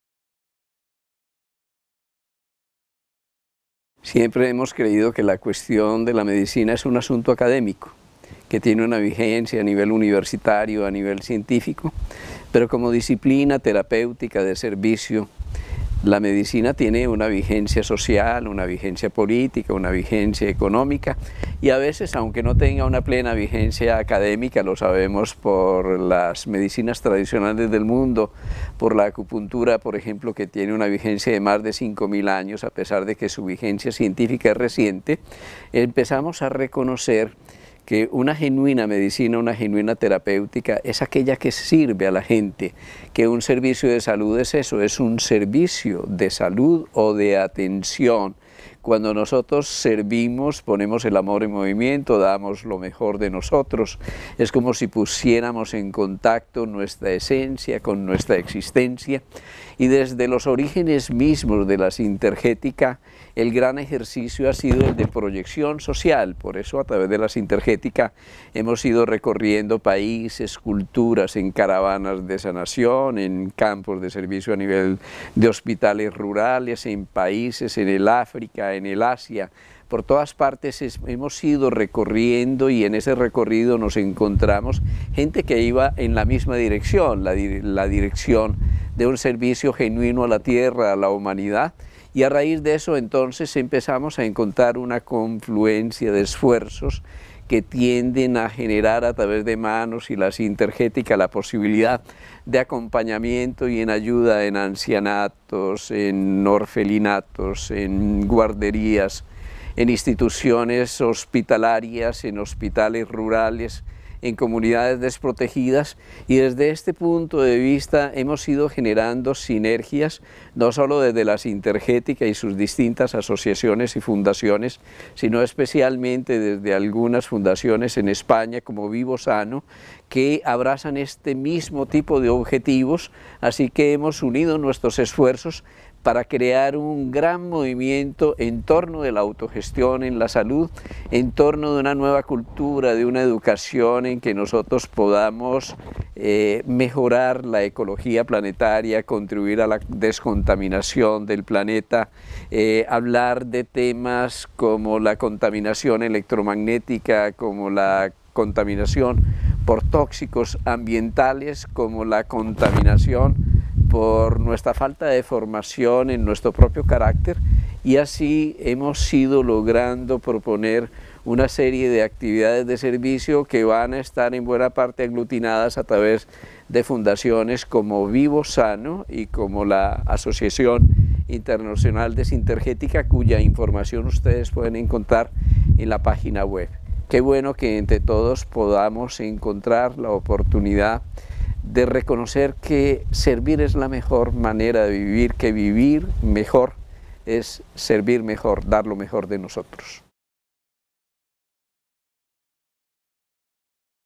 PROYECCIÓN SOCIAL DESDE LA SINTERGETICA. DR JORGE CARVAJAL
En este vídeo el Doctor Jorge Carvajal, creador de Sintergética y de “Manos para Sanar” hace una breve explicación acerca de la labor de Proyección Social realizada desde el entorno de la Sintergética.